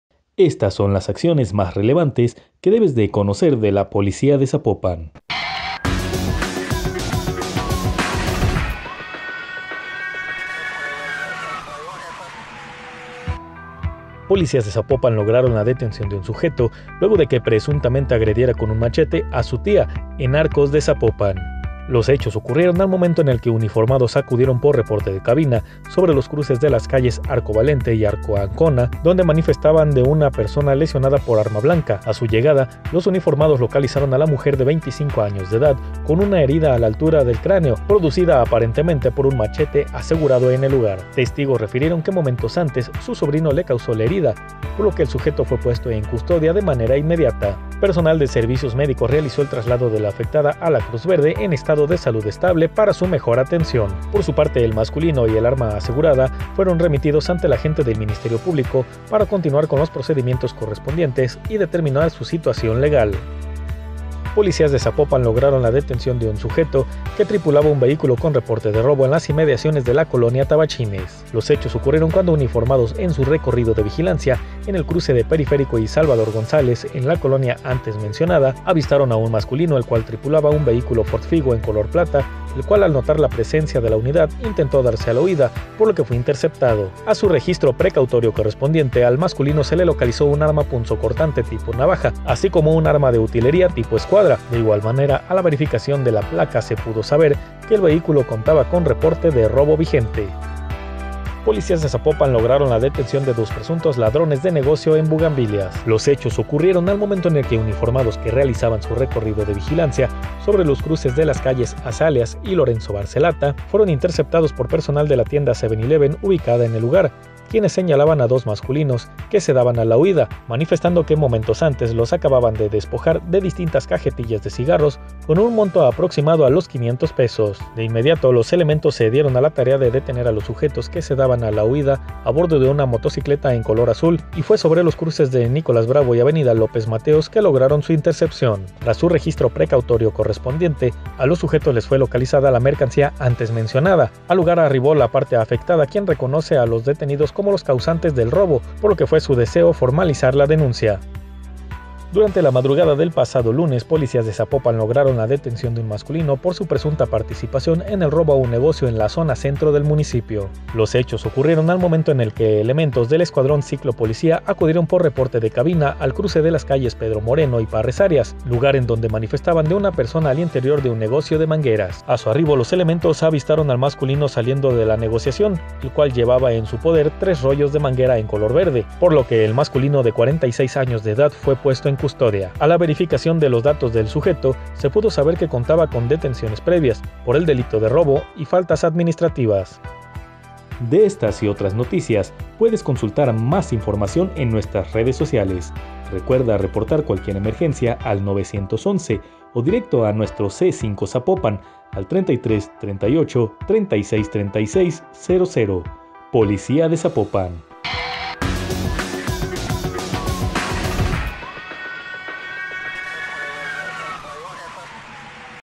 Relevantes Policía de Zapopan 23 de Junio 2020
Entérate de las acciones más relevantes de la Policía de Zapopan en los últimos días. 👮🏻‍♀️👮🏻‍♂️🚓